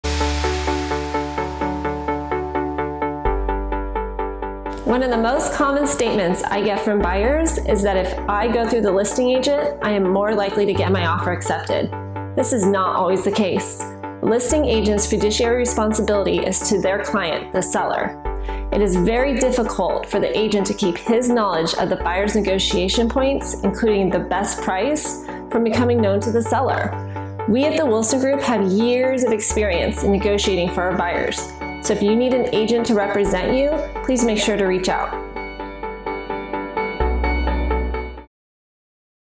Jennifer McCormick - Will I Get My Offer Accepted If I Go Through The Listing Agent?
A series of tips from The Wilson Group to help you with the process of buying a home.